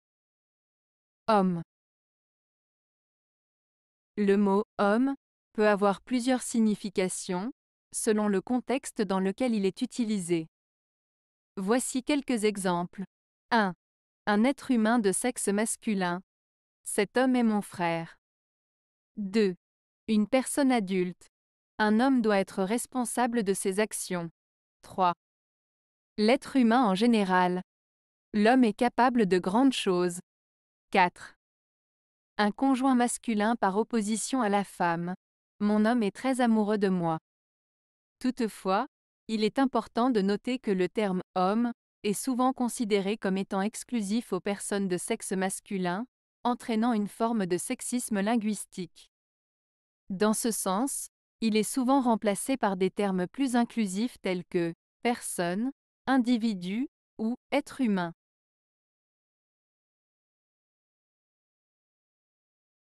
Homme
Le mot "homme" peut avoir plusieurs significations, selon le contexte dans lequel il est utilisé. Voici quelques exemples :
1. Un être humain de sexe masculin : "Cet homme est mon frère."
2. Une personne adulte : "Un homme doit être responsable de ses actions."
3. L'être humain en général : "L'homme est capable de grandes choses."
4. Un conjoint masculin par opposition à la femme : "Mon homme est très amoureux de moi."
Toutefois, il est important de noter que le terme "homme" est souvent considéré comme étant exclusif aux personnes de sexe masculin, entraînant une forme de sexisme linguistique. Dans ce sens, il est souvent remplacé par des termes plus inclusifs tels que "personne", "individu" ou "être humain".